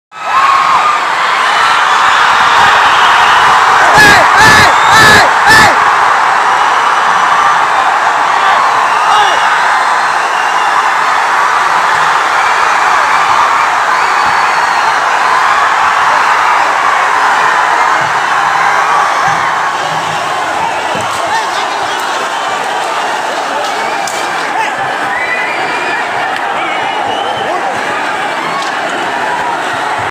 Ilayathalapathy | pondicherry | Sarkar
Marriage function in illayathalapathy .. actor vijay with his wife at pondicherry Most simplest person